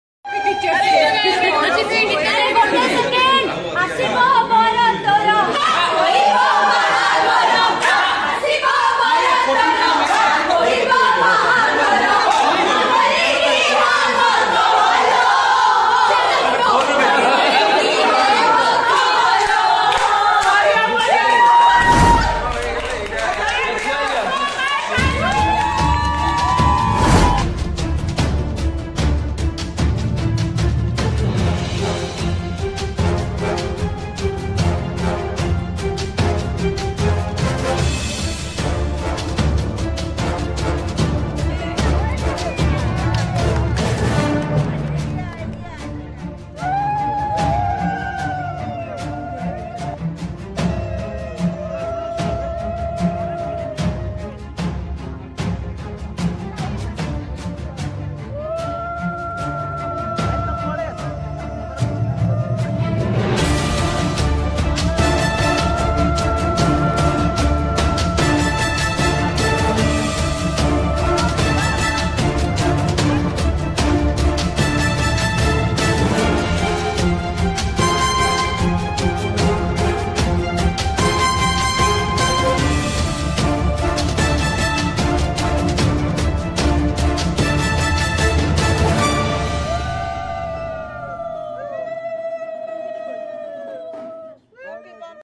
ହାତକୁ ଦି ହାତ ହେଲେ ଓଡ଼ିଆ ସିନେ ଅଭିନେତ୍ରୀ ରିୟା ଦେ
ସରିଲା ଅଭିନେତ୍ରୀ ରିୟାଙ୍କ ବାହାଘର@MMNEWSODIA